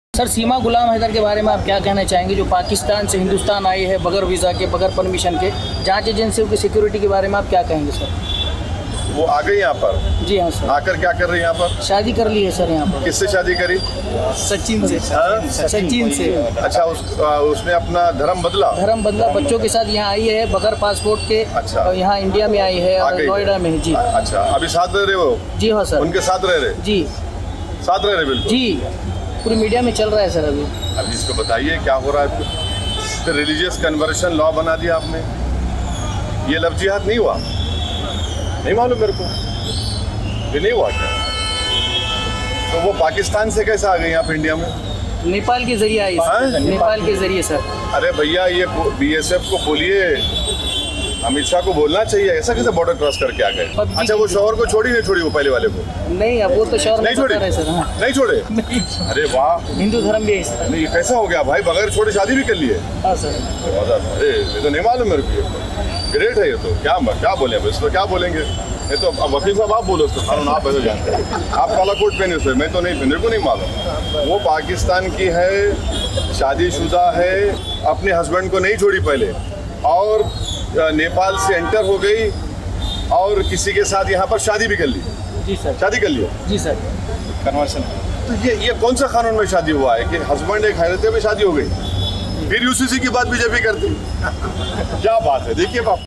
Seema Haider News: सीमा हैदर पर Asaduddin Owaisi ने ये कैसे सवाल पूछ लिए? | PUBG Love Story
कुछ वक्त पहले पाकिस्तानी महिला सीमा हैदर अवैध तरीके से भारत आई थी. अब उसे और उसके भारतीय प्रेमी सचिन मीणा को लेकर काफी चर्चा हो रही है. इन दोनों को हाल ही में एक अदालत ने जमानत दी थी. इससे पहले सीमा के भारत में अवैध रूप से रहने को लेकर उसे और सचिन मीणा, दोनों को पुलिस ने गिरफ्तार किया था. इस मामले में फिलहाल जांच जारी है.